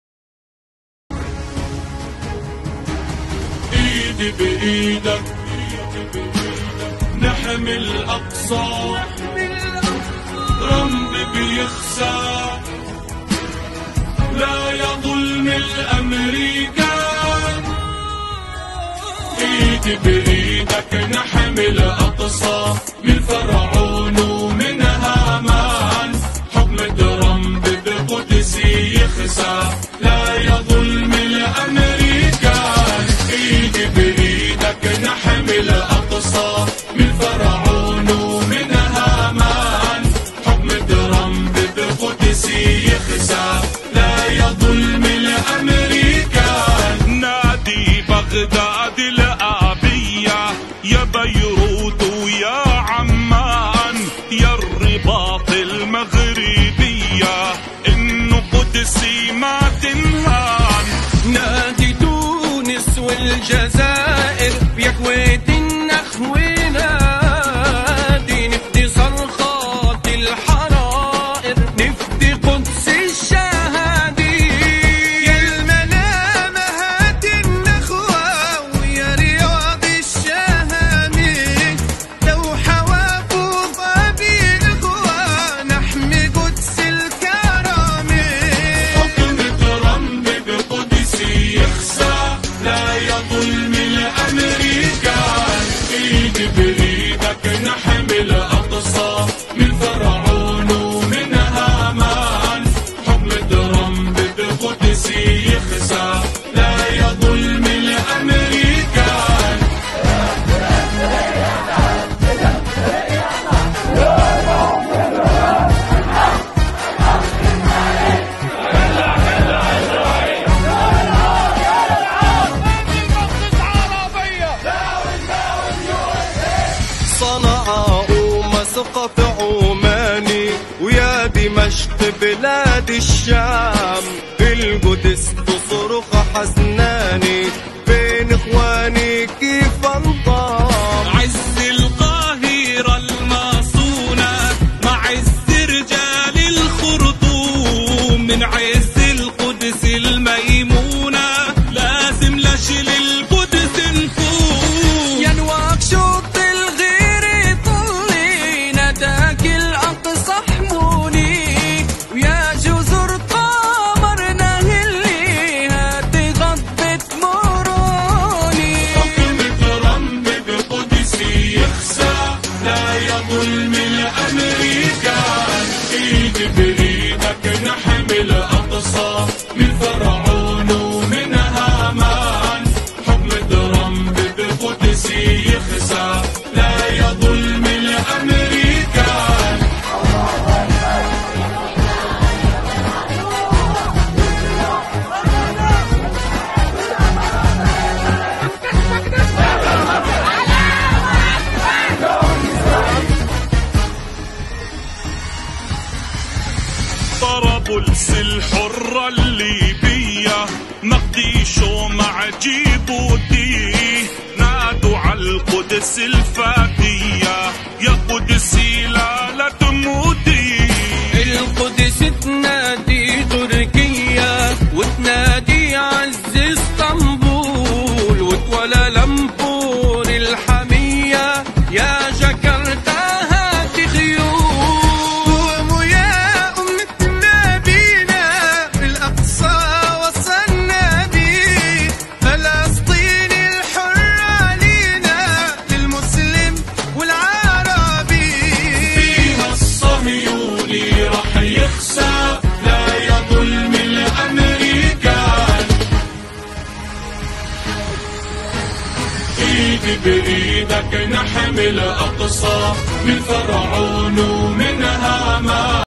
Al Aqsa Menyeru Kuala Lumpur dan Jakarta serta seluruh dunia
Meremang bulu roma bila jakarta dan kuala kumpur di seru membela al aqsa.